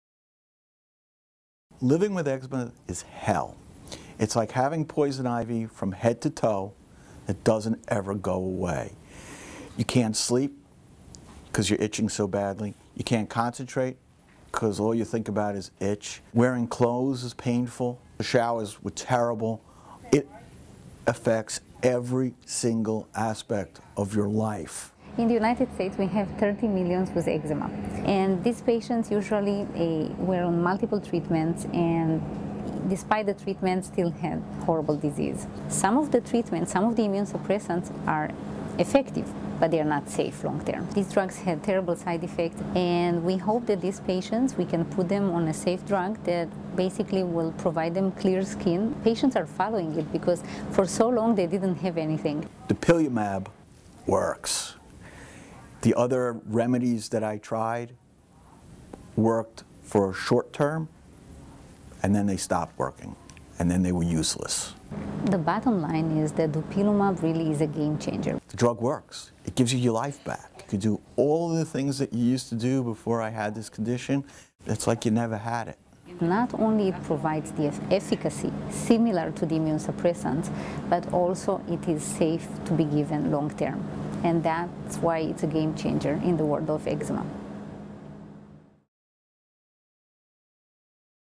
Game-Changing Drug Gets FDA Approval For Eczema
An eczema patient in a Mount Sinai clinical trial and a Mount Sinai physician-scientist who led the trial discuss Dupixent® (dupilumab), the first biologic medicine approved by the U.S. Food and Drug Administration for the treatment of adults with moderate to severe atopic dermatitis. The drug was fast-tracked because it is a “breakthrough therapy” for a large population of adult patients who have uncontrolled AD and need safer and more effective long-term treatments.